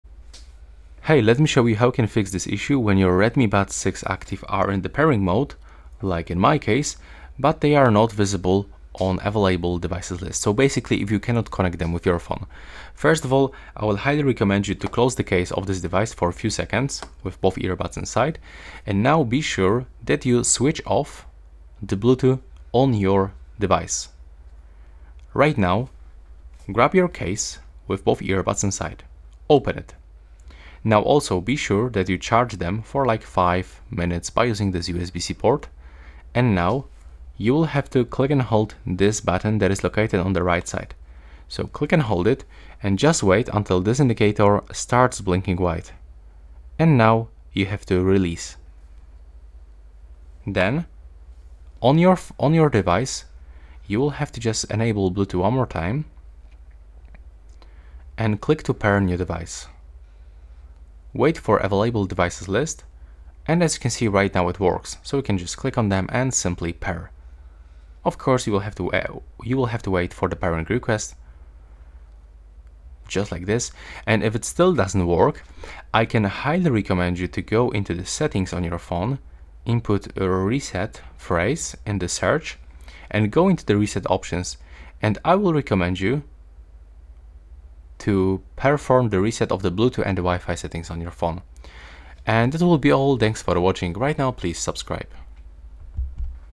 Redmi Buds 6 Active: How to Fix Connection Problems
Find out how to fix connection problems with the Redmi Buds 6 Active. This video walks you through troubleshooting steps to resolve connectivity issues and ensure a stable connection with your Redmi Buds 6 Active. Enjoy seamless audio once again!

I'll do my best to respond quickly and help you out.